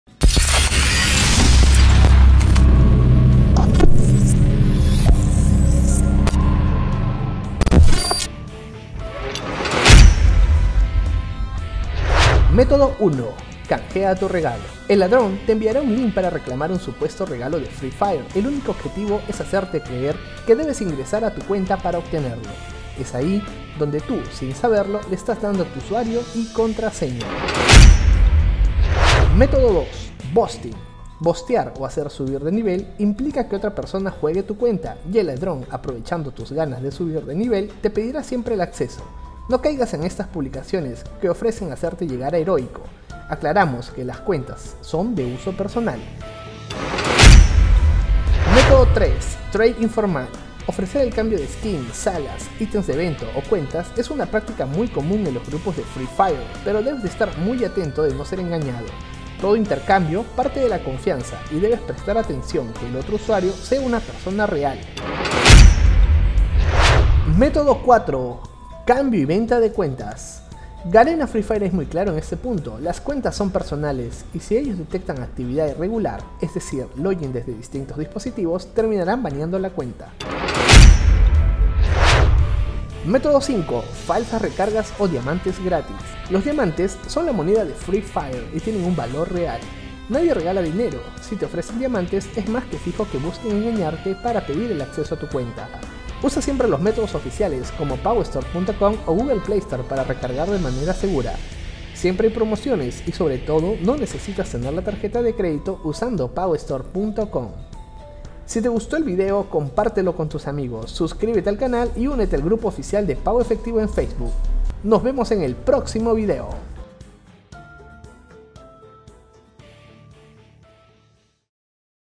[FreeFire Report] 5 métodos que usan para robar tu cuenta de FreeFire
¡Bienvenido al segundo episodio de FreeFire Report!

►¡No olvides ingresar a y recargar tus diamantes con PagoEfectivo